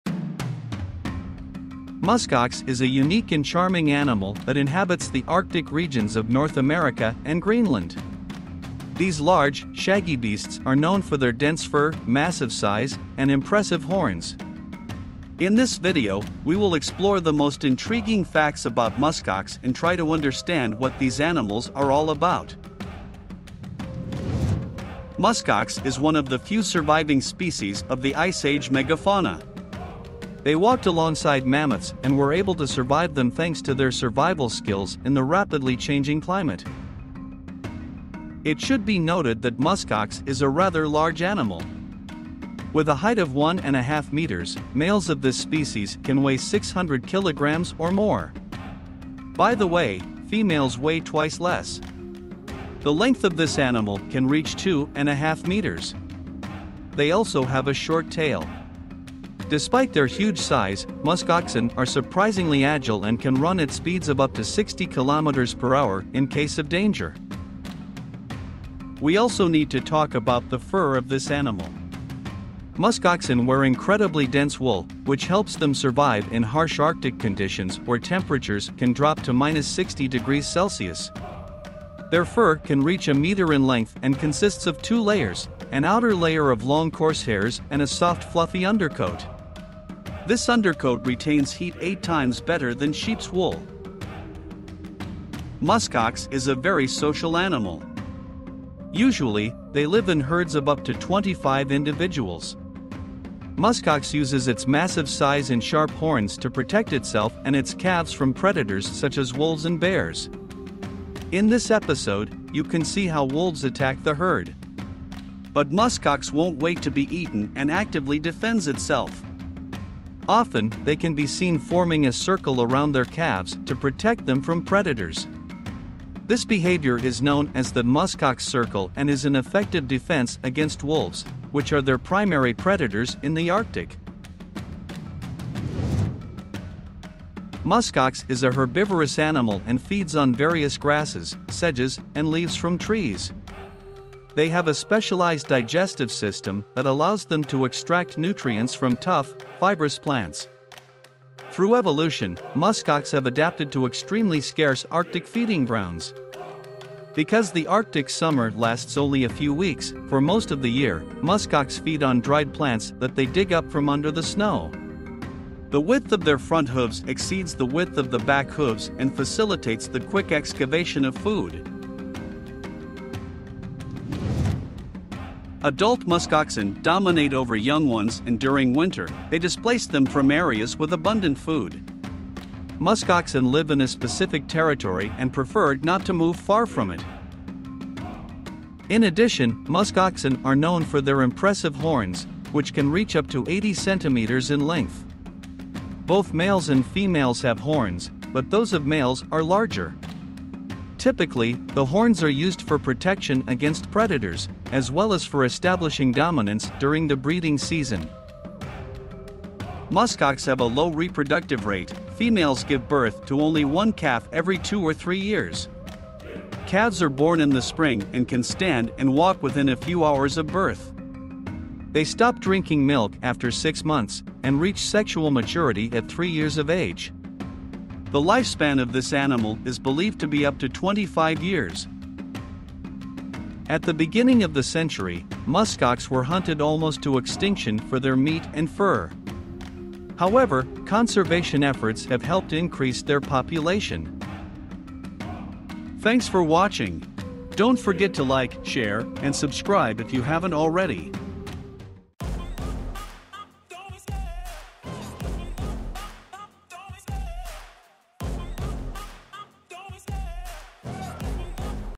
Muskox: The Kings of the Arctic Plains
As the wind howls across the Arctic tundra, a mighty creature roams free. It stands tall, with a thick coat of fur and sharp, curved horns, ready to face whatever challenges the unforgiving wilderness throws its way. This is the muskox, an icon of the Arctic and a symbol of survival in one of the harshest environments on Earth. Join us now as we embark on an epic journey into the heart of muskox country, where we'll witness the breathtaking beauty and power of these magnificent beasts, and discover the secrets of their remarkable survival in a world where only the toughest can thrive.